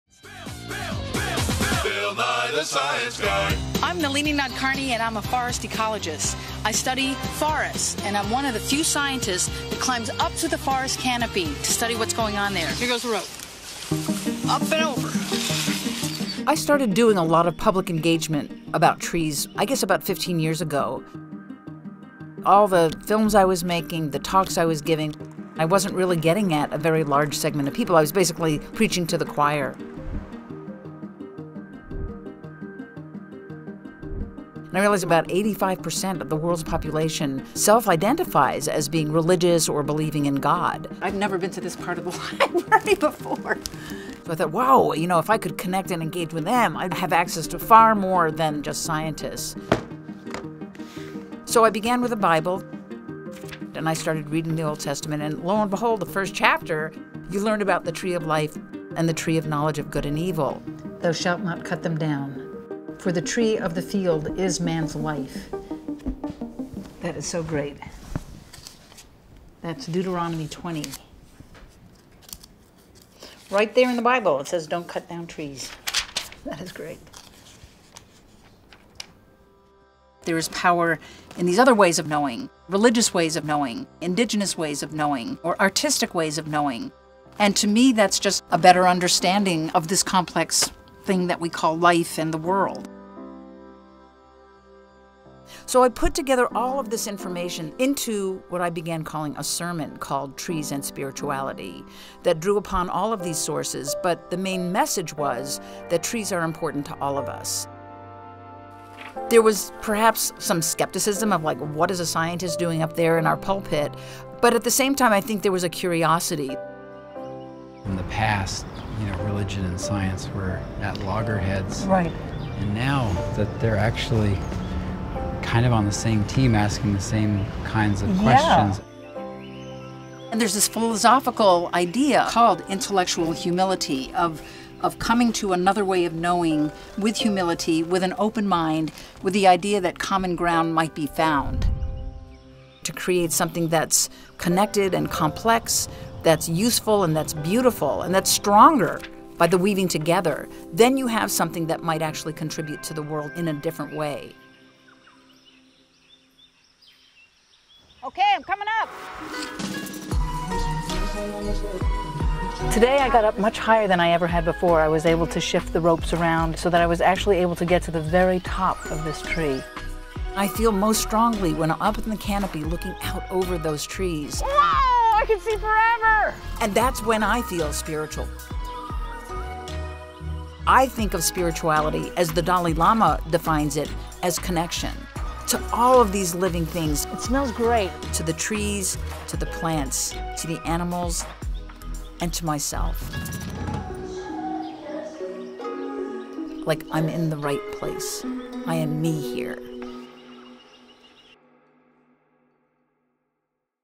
Dr. Nalini Nadkarni | Intellectual Humility Part 1: Religion & Science
Ecologist Dr. Nalini Nadkarni, often called the Queen of the Canopy, is a trailblazer in forest research and an inspiration behind Mattel’s STEM Barbies. Nalini believes that a scientist’s duty is to reach as many people as possible, fostering a deeper understanding of the world and our place in it.
After decades of sharing her research with the scientific and academic communities, Nalini began connecting with new audiences. She developed an ethos of Intellectual Humility, recognizing the value of diverse ways of knowing beyond science. By meeting people where they are, she identifies shared values around conservation and nature.

Featuring Dr. Nalini Nadkarni

Directed By Andrew P. Quinn

Production Services Bayonet Media
Post Production Services Chop Shop Post

Producers
Jake Foster
Andrew P. Quinn

Consulting Producer
Eben Hall

Executive Producers
Andrew P. Quinn
Joe Vella
Kevin Grazioli

R. Aaron Webster

Edited By
Amelia Kramer
Post Production Supervisor
Hannah Lindgren

1st AC
Kolton Dallas

Director of Photography Montana Unit
Brian Hollars

Sound Mixer
Josh Norton

Special thanks
The University of Utah